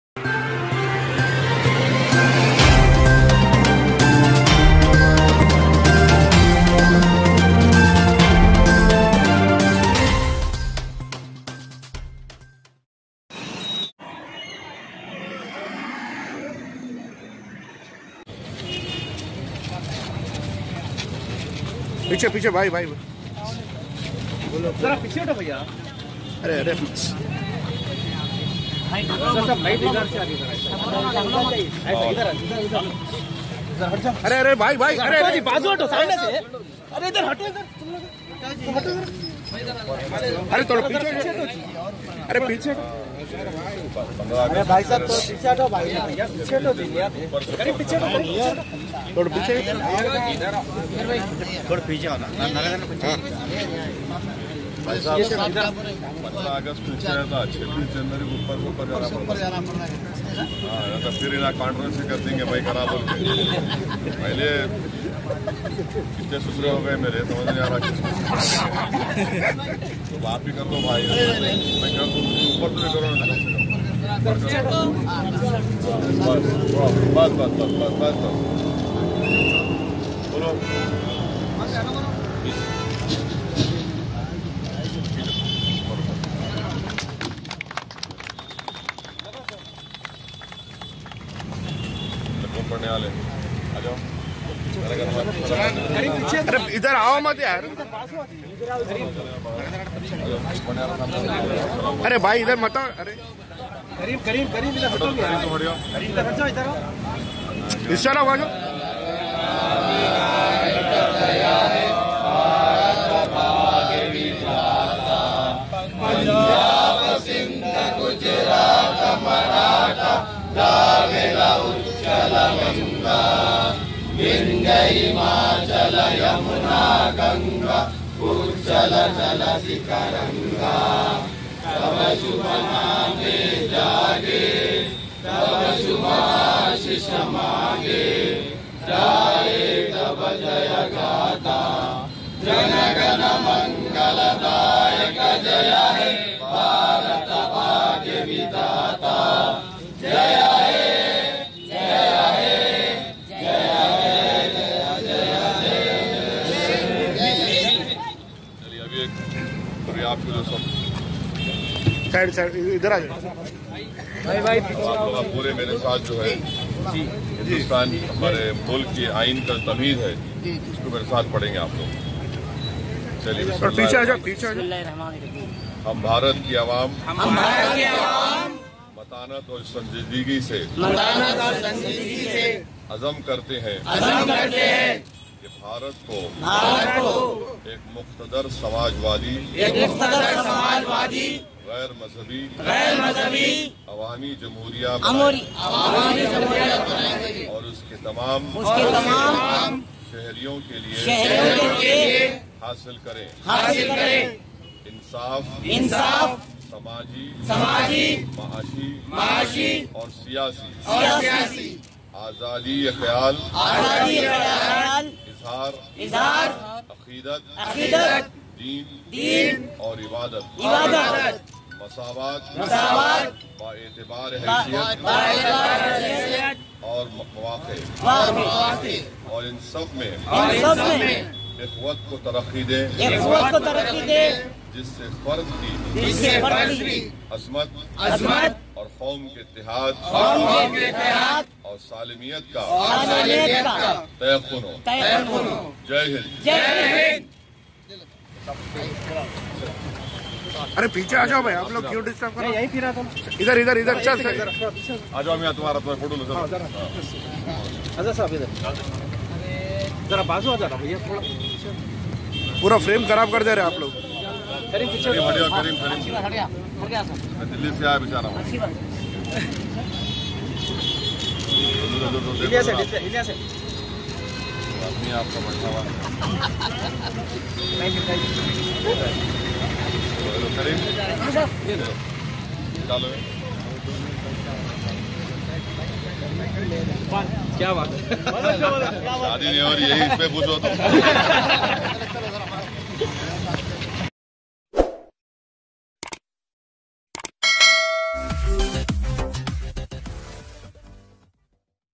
News53| AIMIM President Barrister Asaduddin Owaisi Sahab Hoists The National Flag 🇮🇳 @ Madina 'X'
AIMIM President Barrister Asaduddin Owaisi Sahab Hoists The National Flag 🇮🇳 @ Madina 'X' Road On 71st RepublicDay..

Report  By Saifuddin Taiyebi